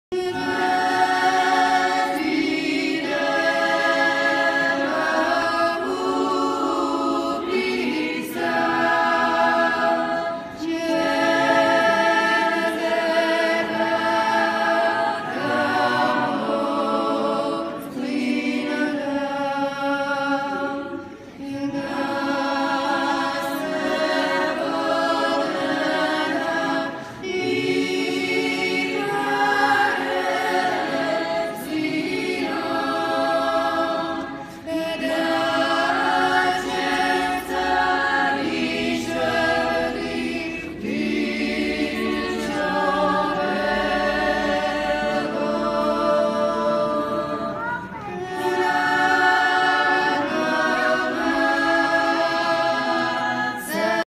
ღვთისმშობლის ხელთუქმნელი ხატის ასლი ამაღლების ტაძარში
დღეს, 12 მაისს, გვიან საღამოს, ახალციხეში, ამაღლების ტაძარში ღვთისმშობლის ხელთუქმნელი ხატის ასლი მიაბრძანეს